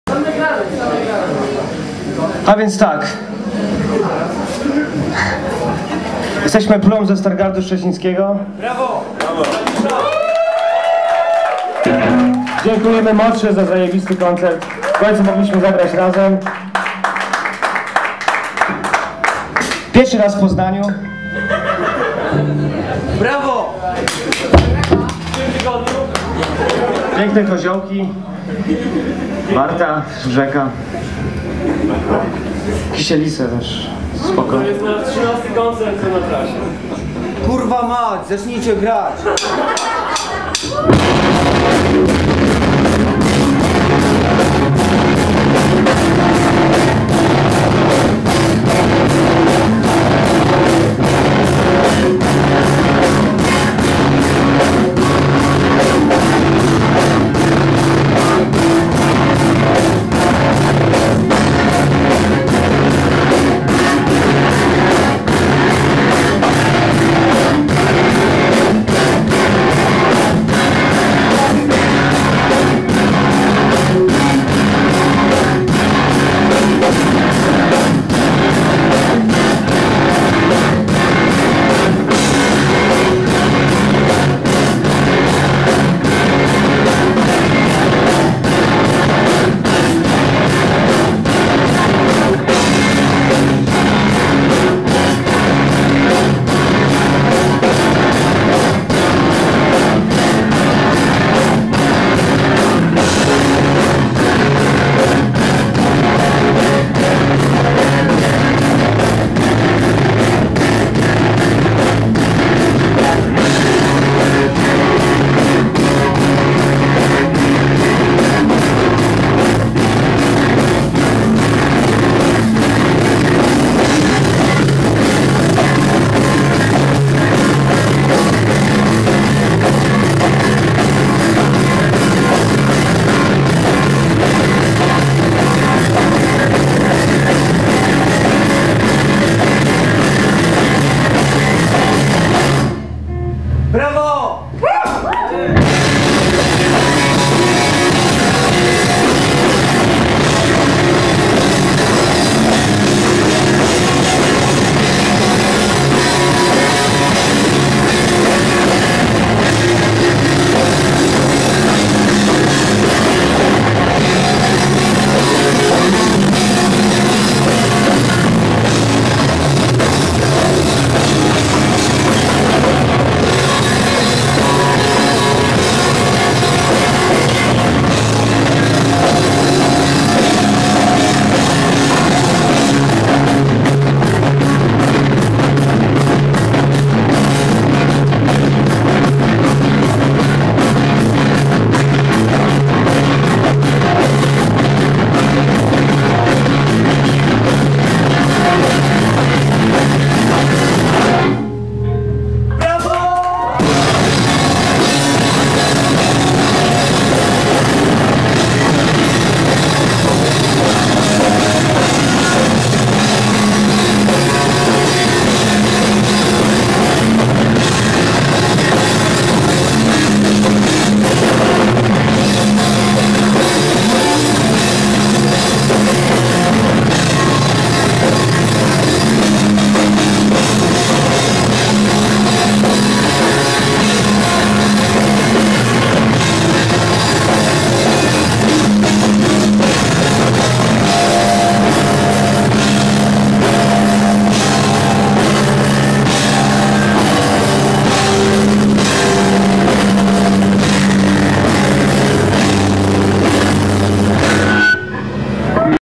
PLUM POZNAŃ 2009 POD MINOGĄ
fragment koncertu PLUMa w poznaniu 28 X 2009 pod minogą